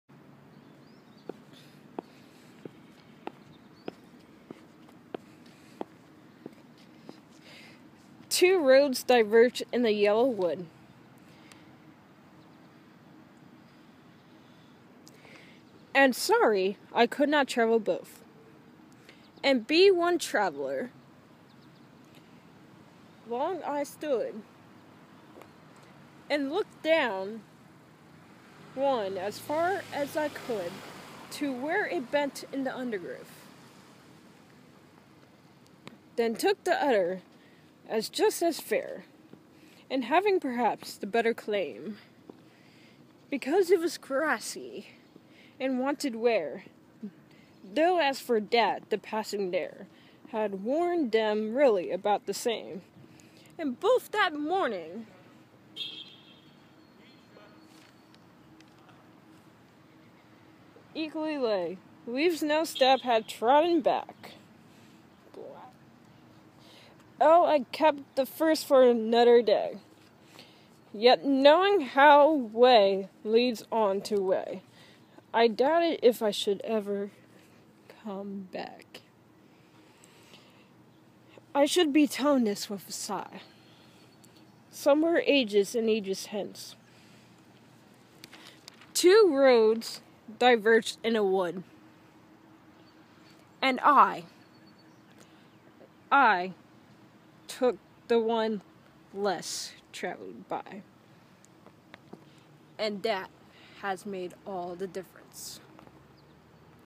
The Road Not Taken
This video is a modern twist on The Road Not Taken by Robert Frost.